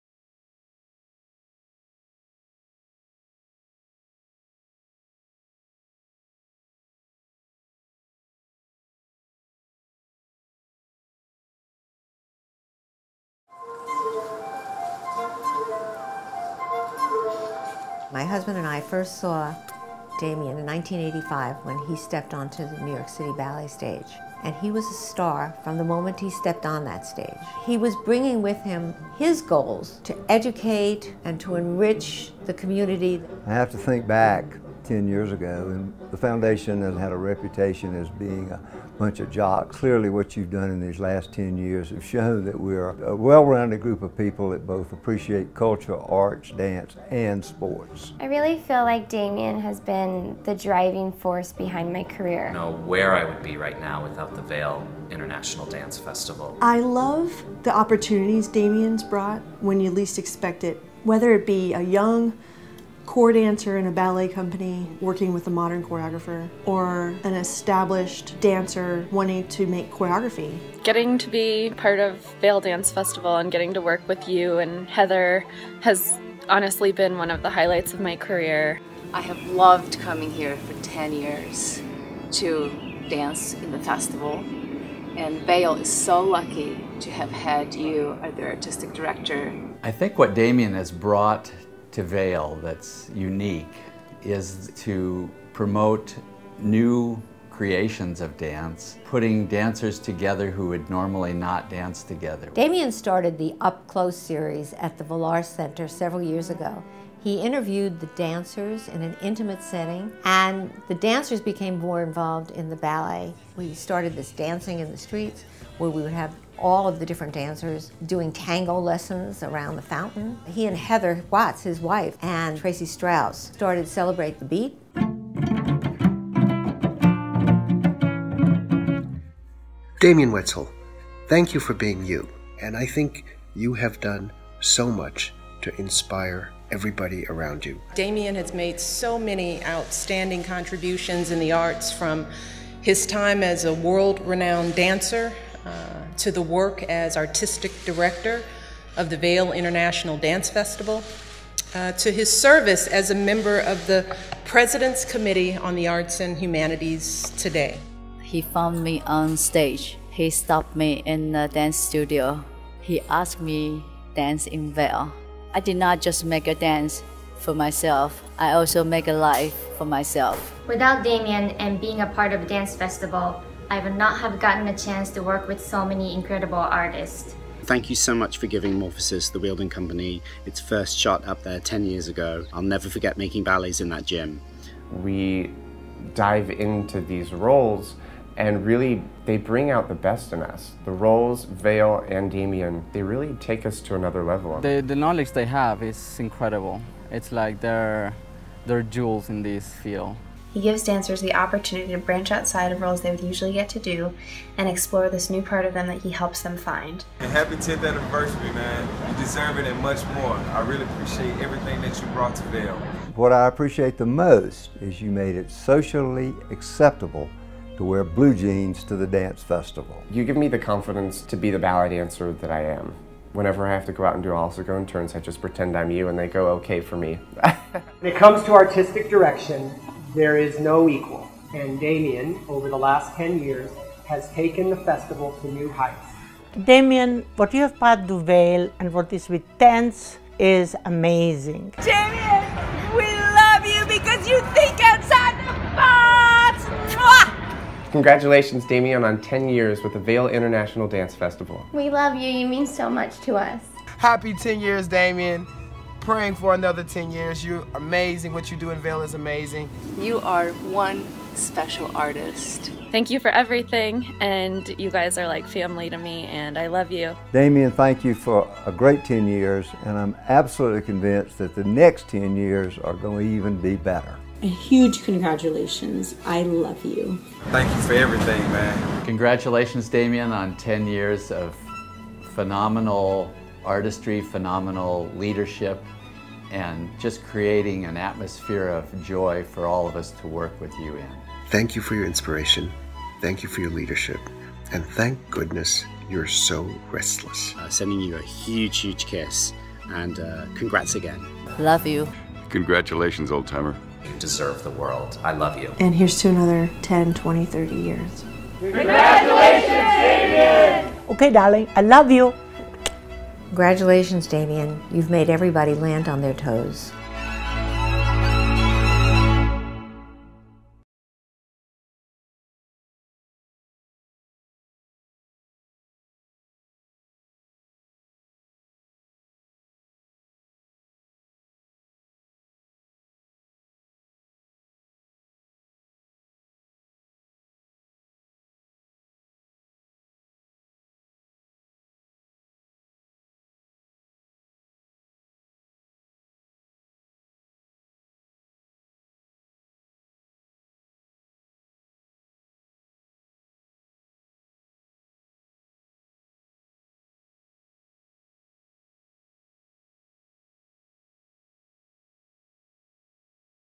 Celebrating 10 Years with Damian Woetzel
With thanks and gratitude to Damian Woetzel for 10 years of artistic excellence.

Video: Nel Shelby Productions

The Vail International Dance Festival is a project of the Vail Valley Foundation.